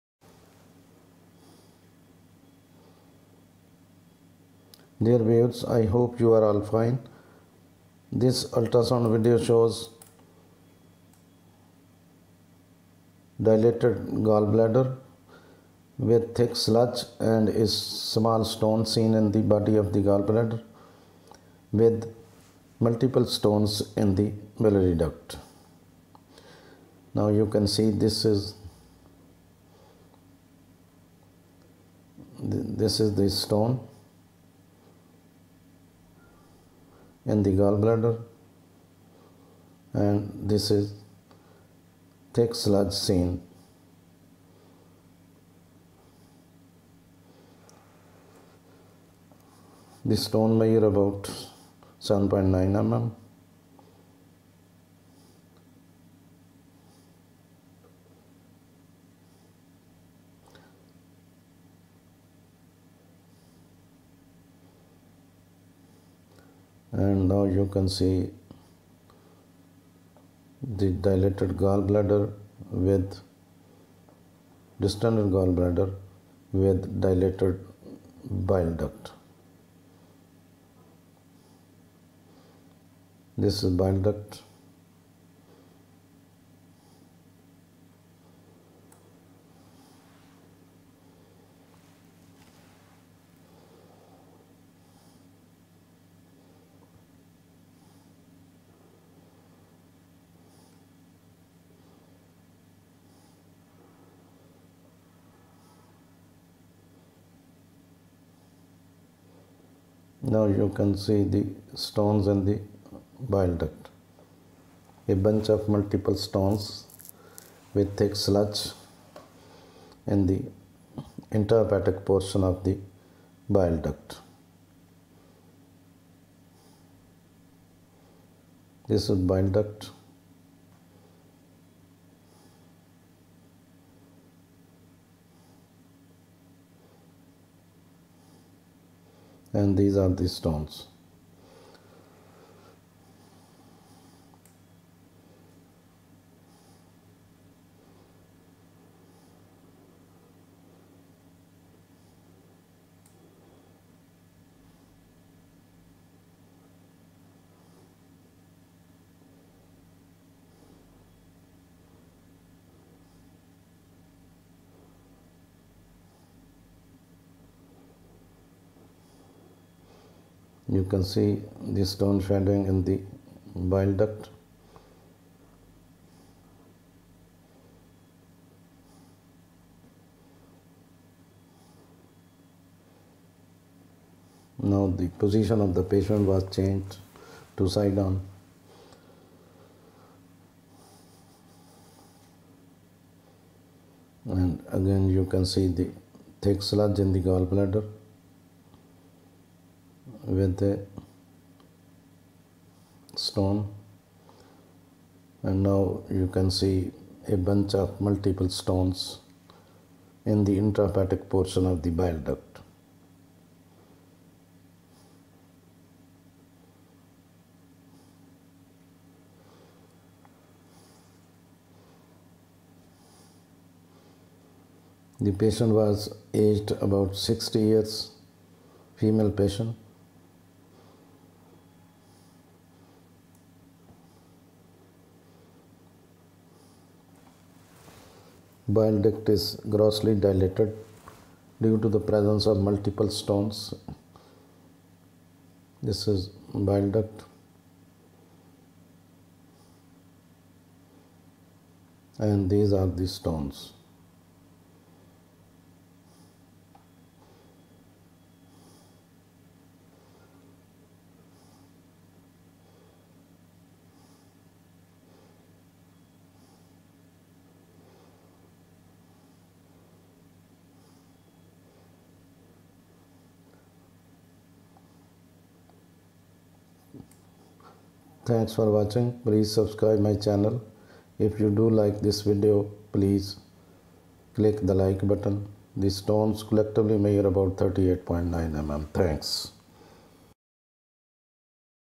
Ultrasound Video showing stone and sludge in Gall bladder with Choledocholithiasis.
This video shows stone and sludge in Gall bladder ( Cholelithiasis ) with Choledocholithiasis.